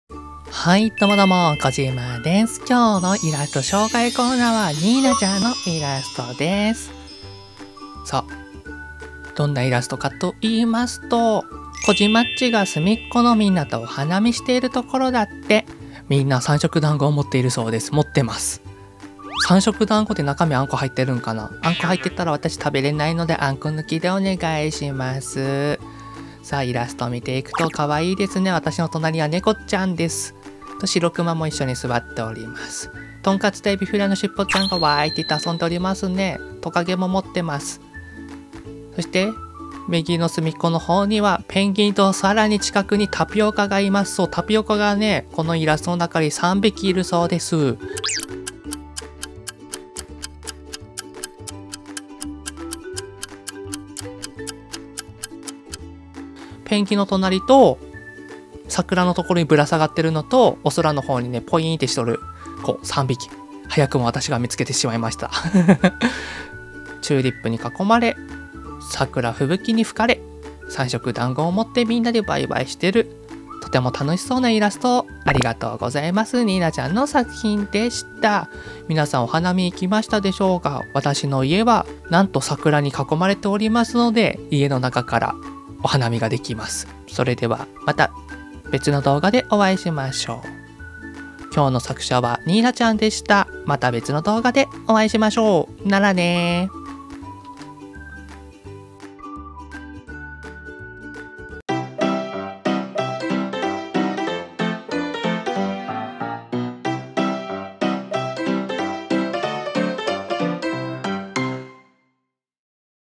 すみっコとお花見🌸イラストもらったよー🖊ぷにっコから届いたよ！にいなちゃんありがとう！
Skype　live:.cid.b7c95d06719c91c

■こじまオリジナルグッズはこちらから(／ω＼)

イラストAC
 PIXTA
OtoLogic